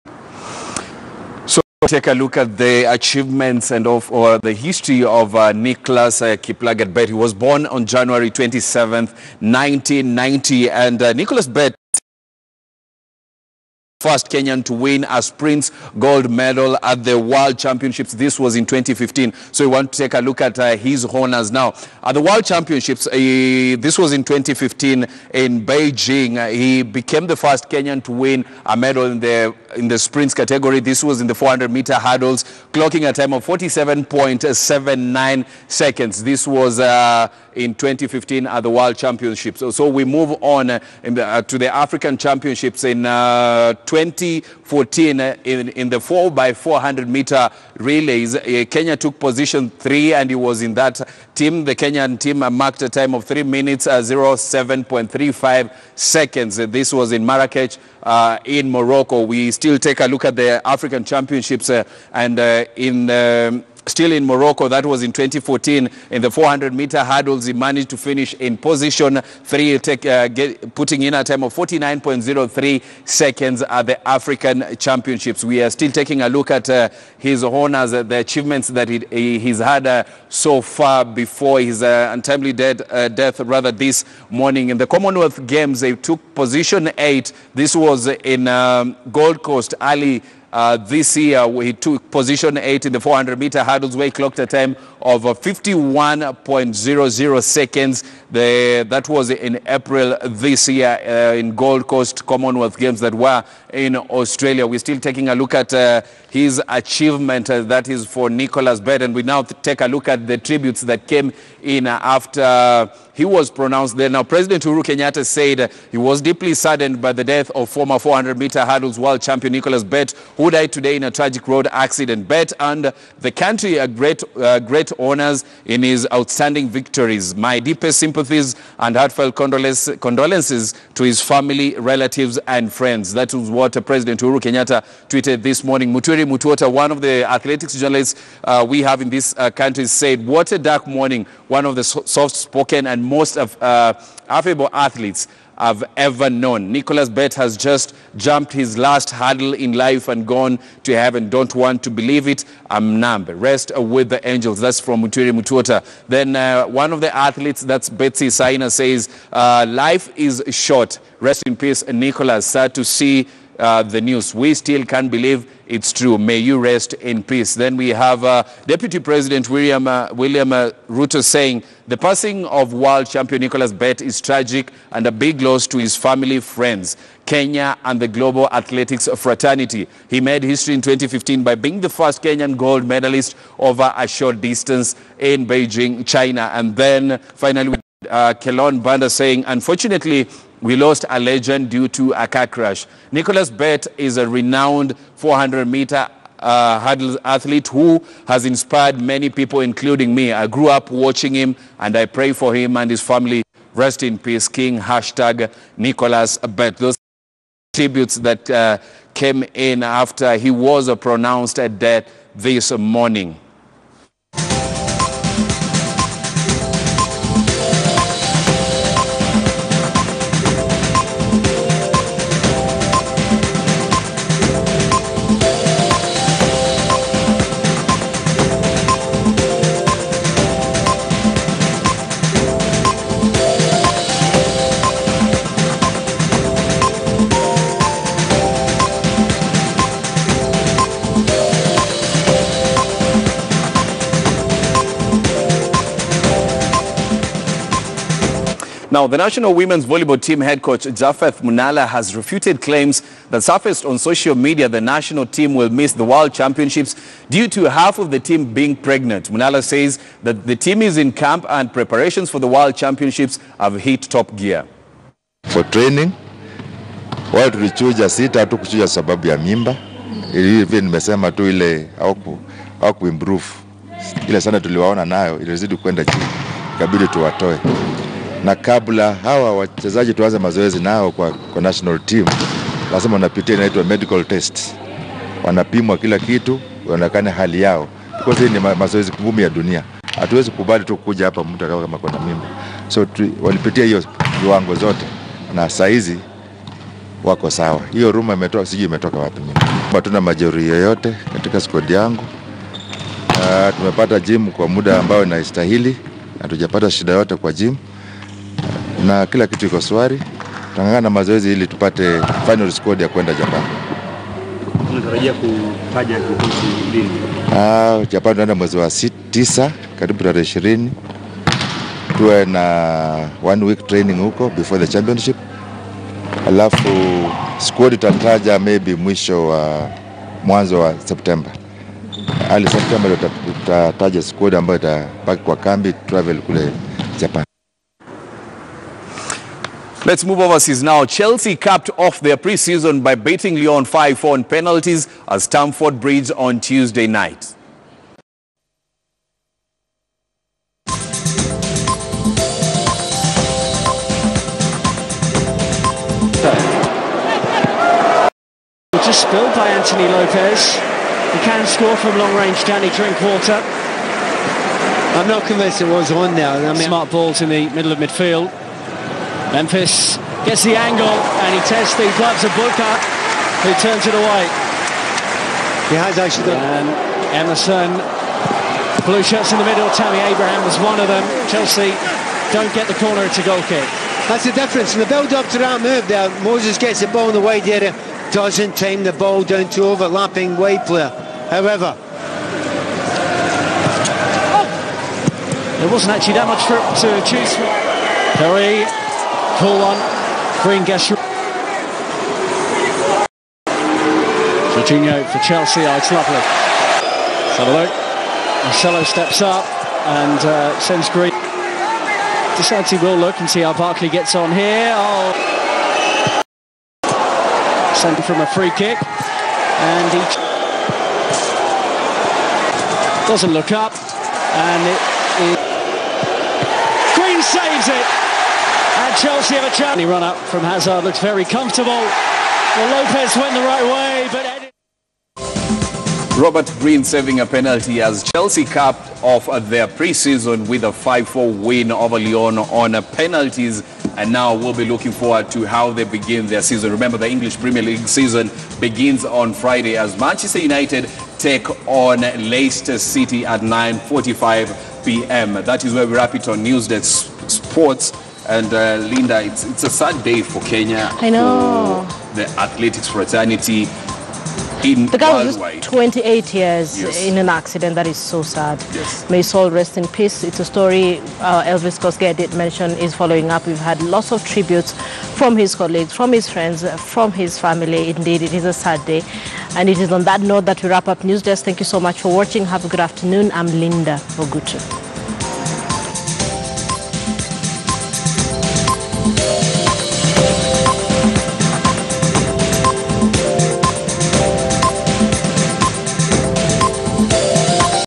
Achievements of World Champion Nicholas Bett
It is a sad day for Kenya following the death of Former 400M hurdles world champion Nicholas Bett's demise. Bett died in a road crash in Nandi this morning, just a day after returning from the Continental Championships in Nigeria.

The athlete is said to have lost control of his vehicle which rolled along the Eldoret-Kapsabet road. Bett made history in China after becoming the first Kenyan to win a gold medal in a short distance race at the world stage. He won the 400m hurdles finals at the 15th IAAF World Championships in Beijing in 47.79 world leading time, beating Russia’s Dennis Kudryavsten in 2015.